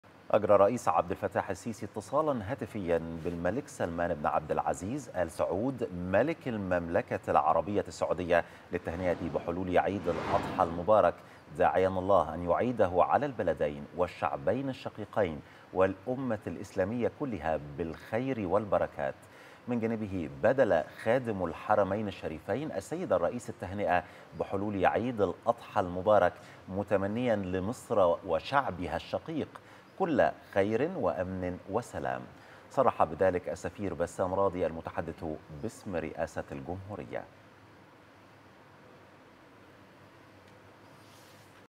الرئيس السيسي يجري اتصالا هاتفيا بالملك سلمان بن عبدالعزيز للتهنئة بحلول عيد الأضحى المبارك
غرفة الأخبار | الرئيس السيسي يجري اتصالا هاتفيا بالملك سلمان بن عبدالعزيز ملك السعودية للتهنئة بمناسبة حلول عيد الأضحى المبارك
19 يوليو 2021